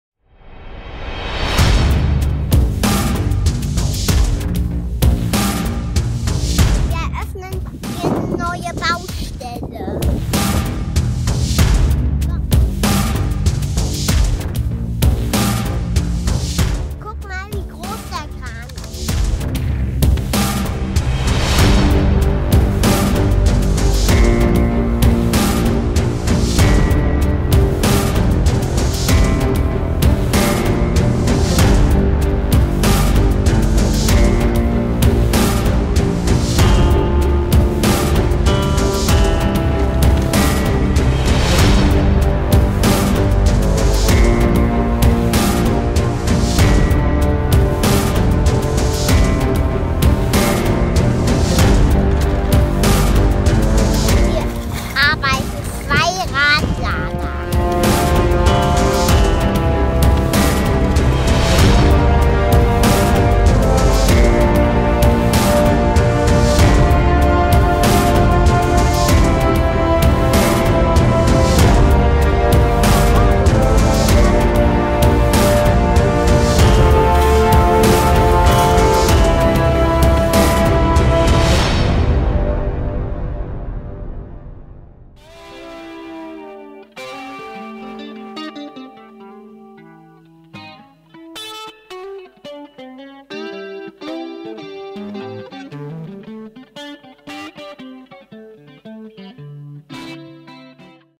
BRUDER TOYS TRUCKS construction HEAVY EQUIPMENT Mining Class TRAILER
Bruder Toys videos shows: Toy Trucks BRUDER bworld Construction - Jack Jack's new opening of a huge outdoor construction site, feat following toys: Bruder 02818 - MACK Granite Liebherr Kran, Bruder 02438 - CAT Schaufelbagger, Bruder 02430 - Liebherr Radlader L574, Bruder 03555 - Scania R-Serie LKW mit Tieflader und Cat Bulldozer, TONKA Dump Truck Mining Class and many more like Bruder 02452 - CAT großer Kettendozer;
Pete Ruppert Toy UNIVERSE; allGlory2theLORDjesus!